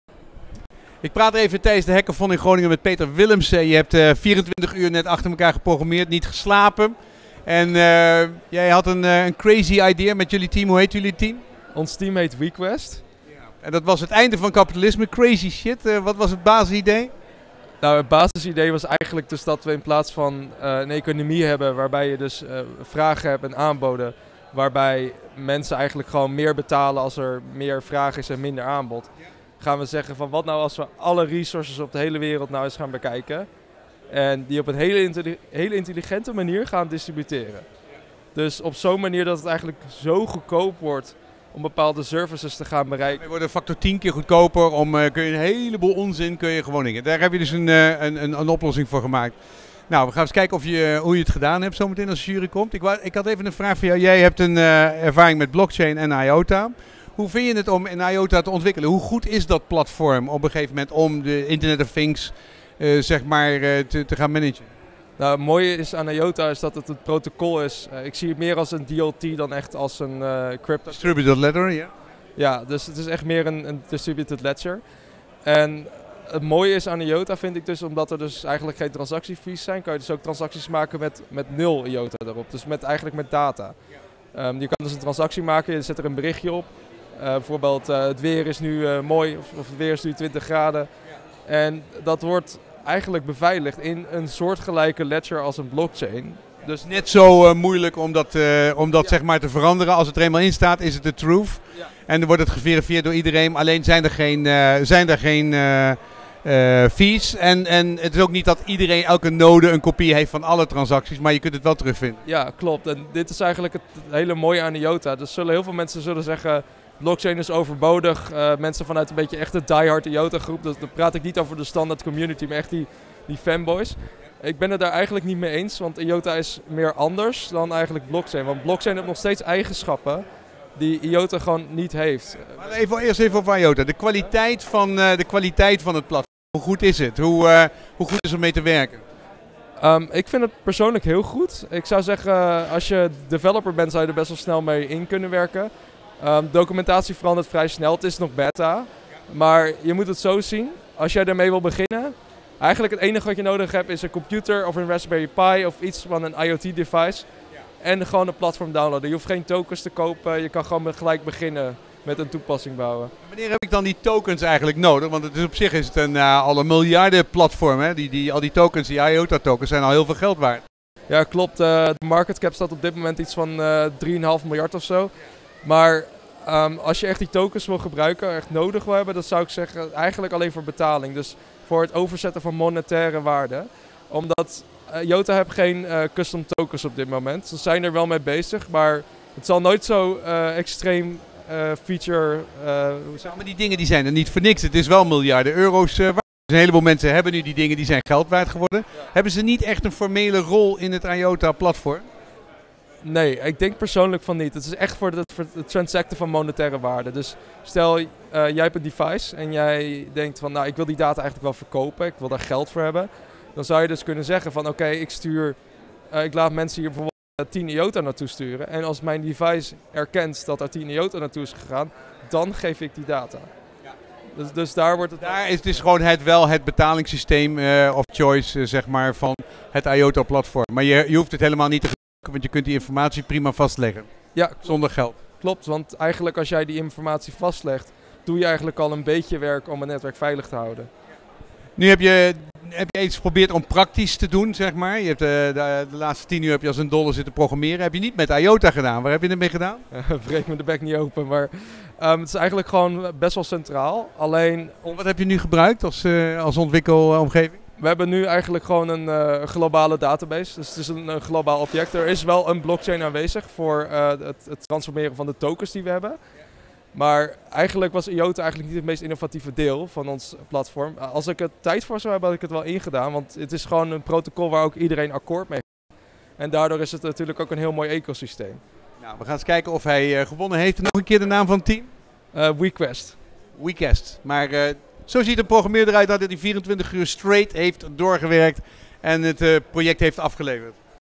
Peter Willemsen (WeQuest) na 24 uur programmeren over IOTA
Ik was op de Blockchaingers Blockchain Hackathon 2018 in Groningen en ik kwam daar Peter Willemsen tegen, programmeur en onderdeel van het WeQuest team. Toen ik Peter tegen kwam was hij erg moe omdat hij net 24 uur achter elkaar door had geprogrammeerd. Ik praat met hem over de kwaliteit van het IOTA platform en wat voor een concept ze hebben verzonnen om de wereld te bevrijden van het kapitalisme. De opdracht was om een crazy idee te creëren en dat hebben ze ook gedaan!